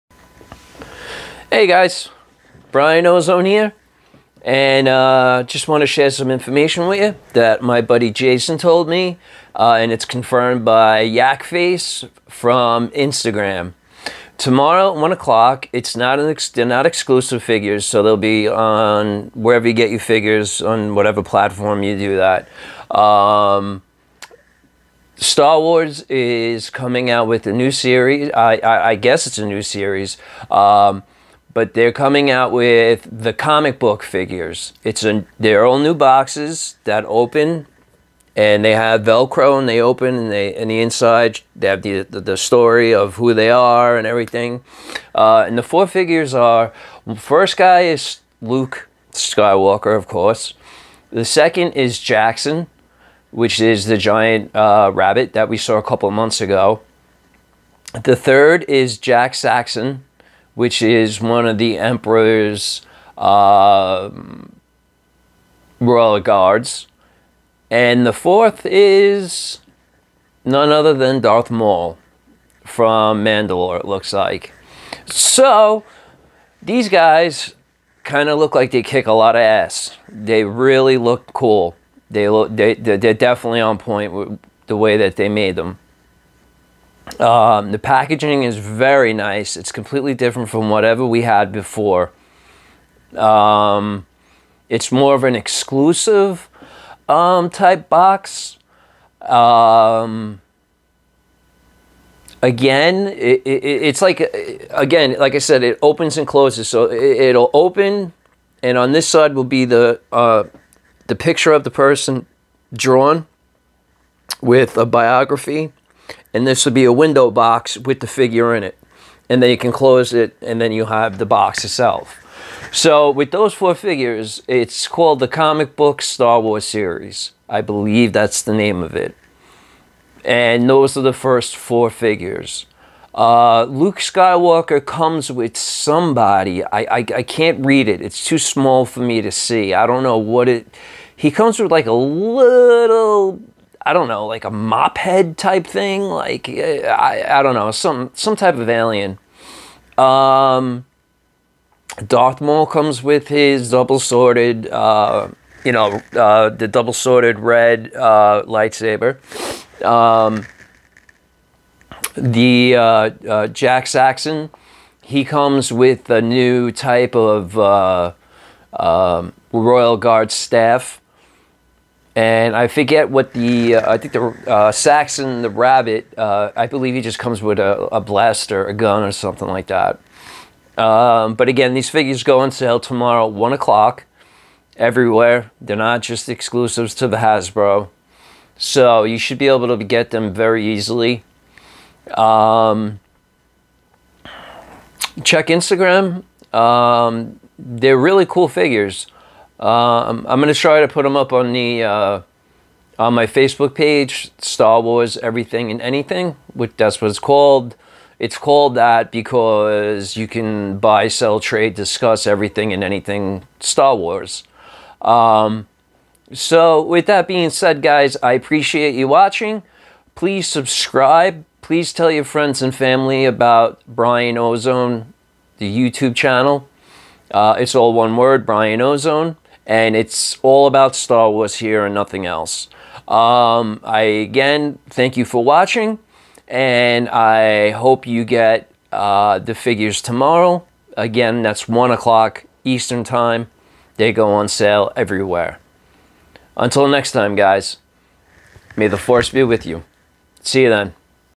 Mar18,21 New Black Series figures come out tomorrow
Jaxon. Luke Skywalker. Darth Maul. Dar Saxxon